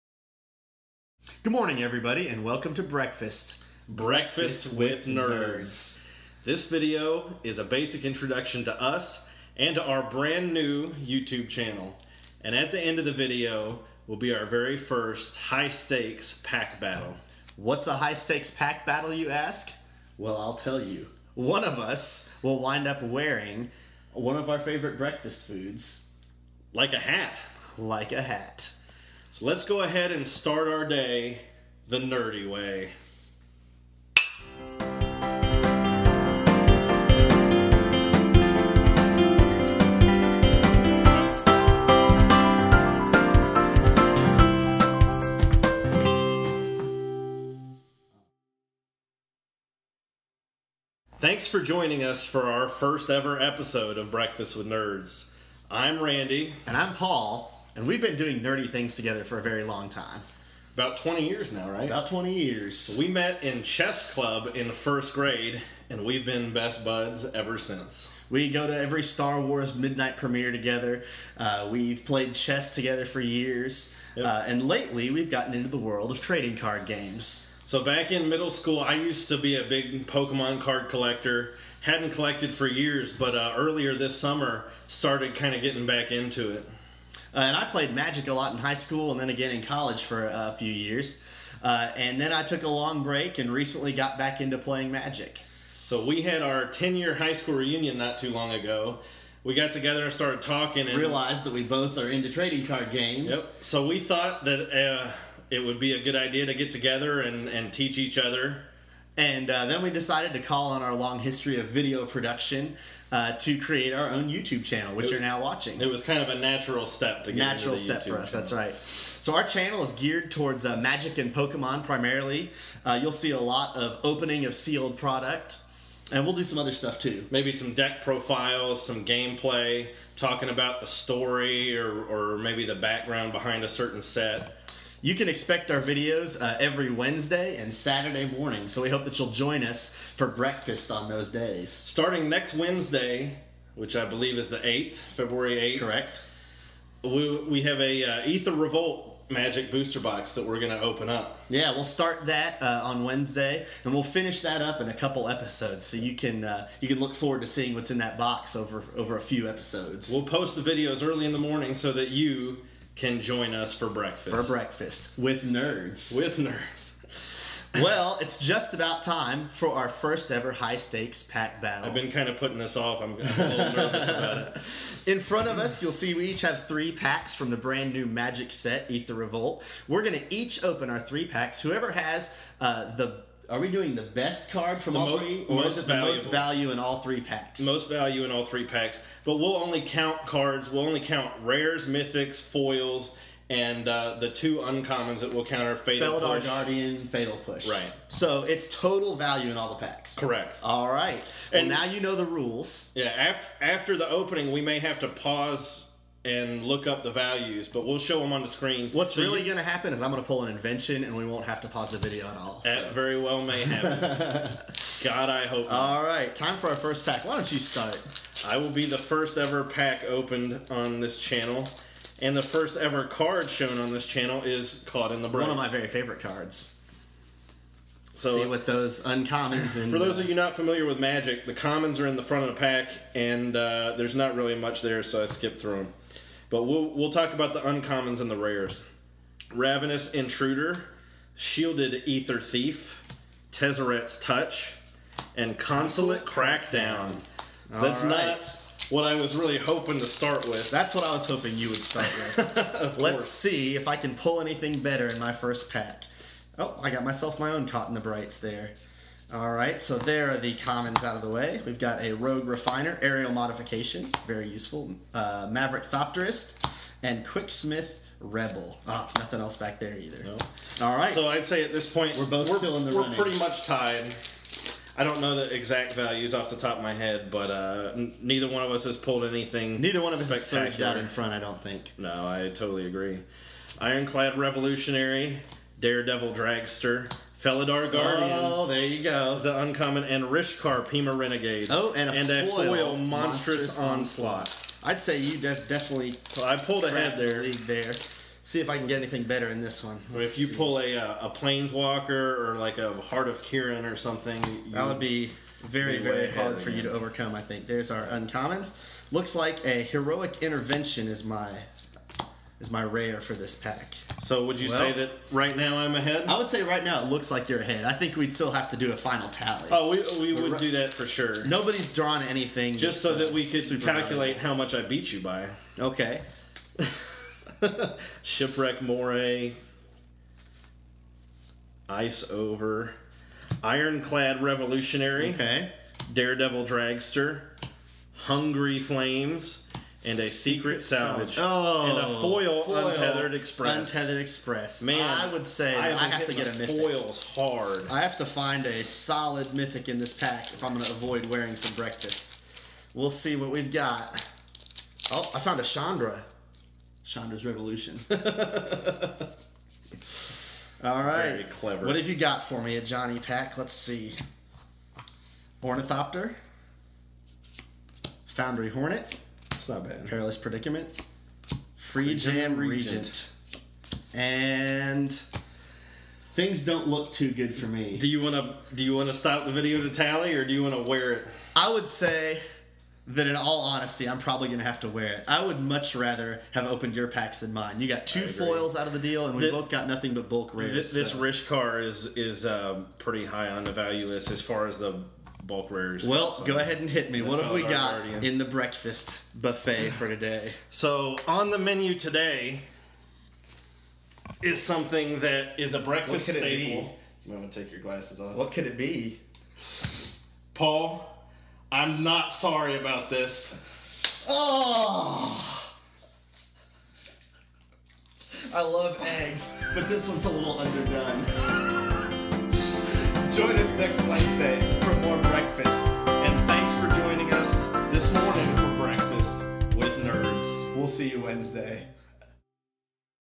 Original Channel Trailer and FIRST High Stakes Pack Battle!
Start your day the nerdy way with us!

This video is a brief introduction to us and our new YouTube Channel.  And at the end of the video, one of us will end up wearing our breakfast.